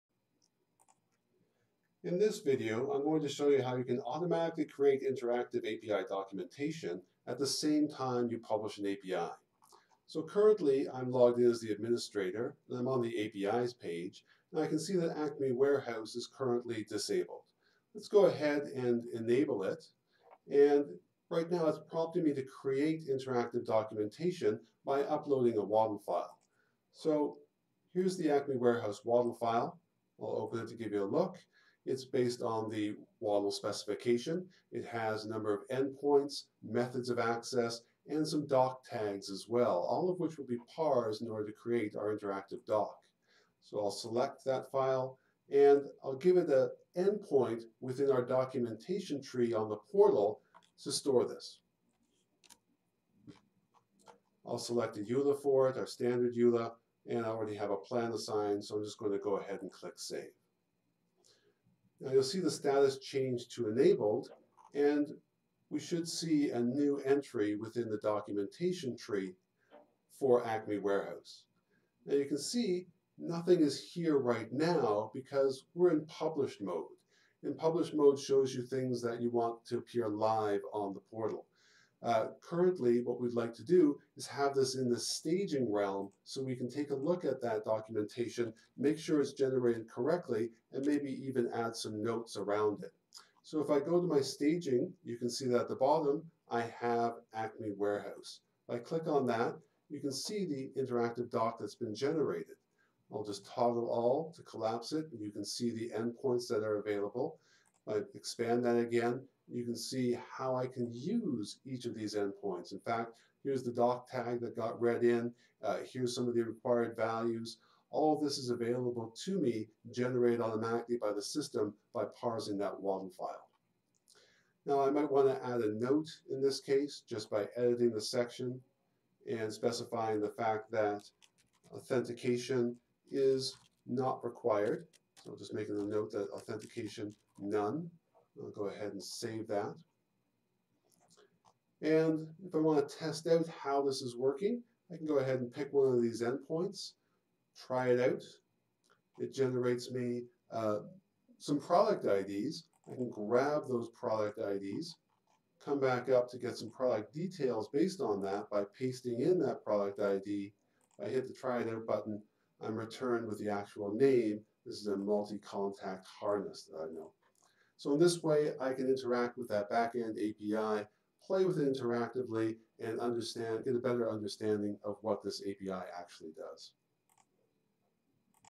Publish Interactive API Documentation Using the Layer 7 API Portal
Publish Interactive doc - this video explains how you can automatically create interactive API documentation at the same time that you publish an API